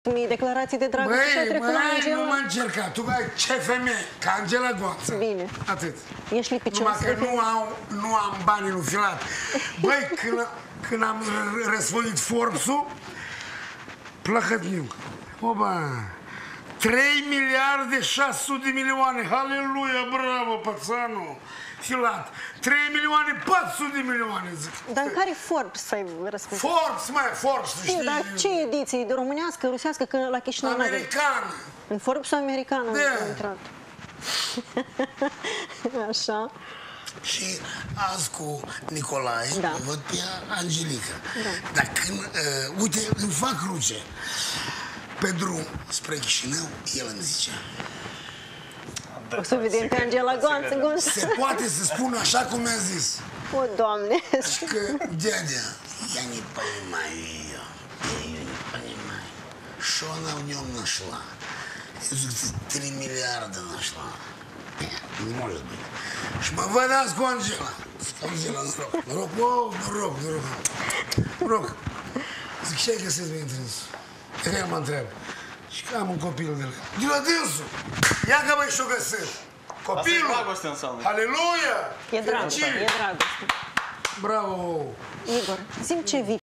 Igor Caras-Romanov, despre secretul dragostei lui Vlad Filat și Angela Gonța
Sursa. Acasă TV